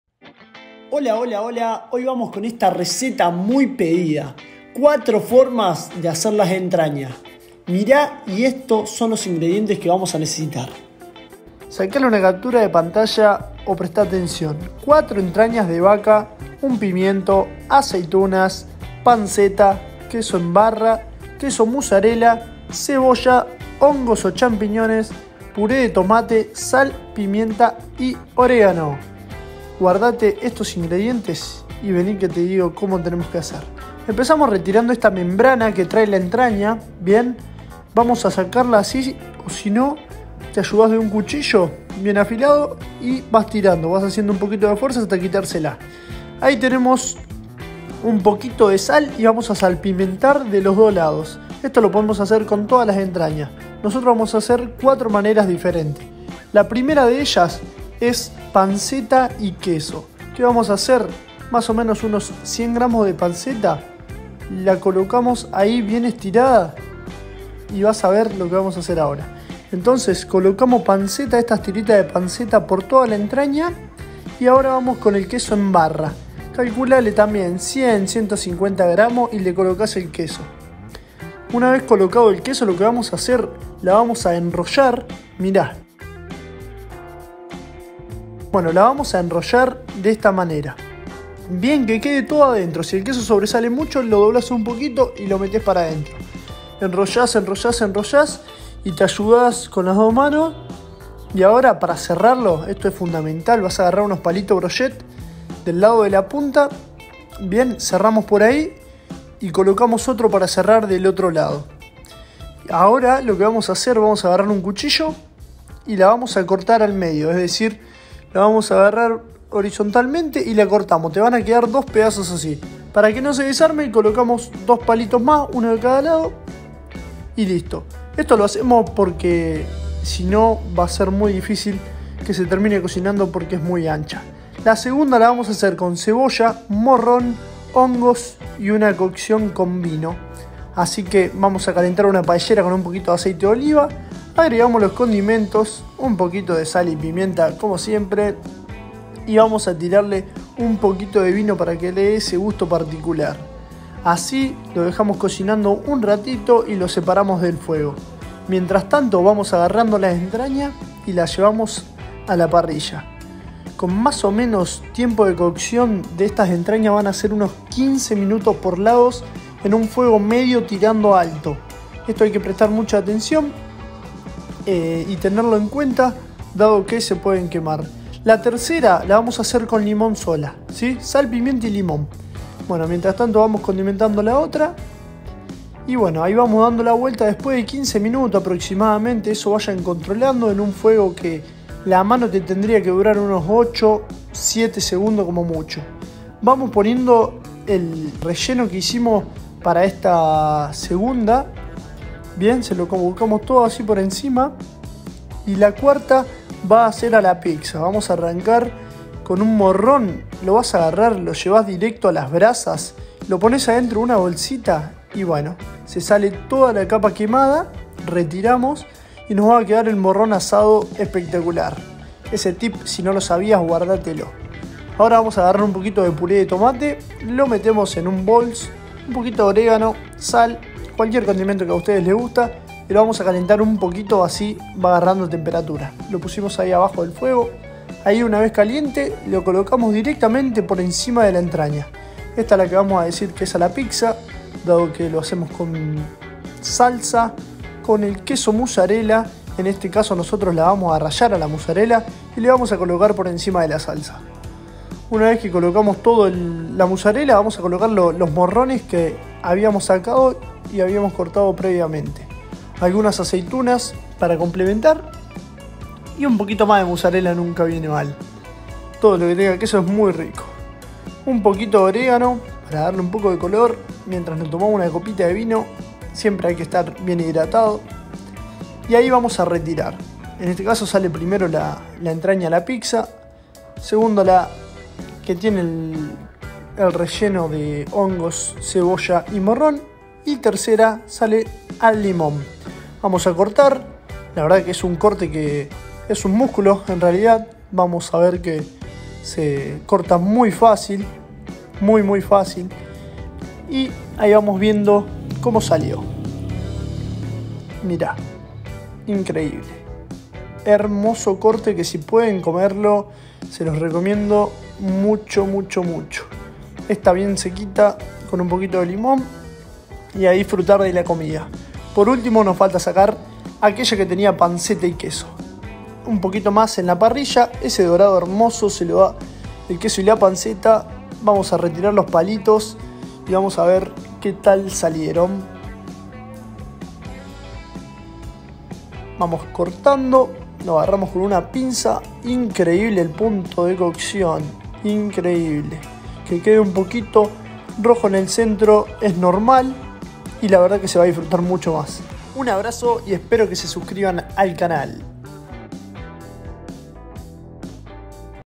Entrañas: 4 formas de hacerlas 😱 | Parrillas Argentinas
Tremendo video!! ENTRAÑAS 🔥🔥 4 formas de realizarlas..

Que te parece ?? Que otra forma te gusta hacerlo?

Somos PARRILLASARG.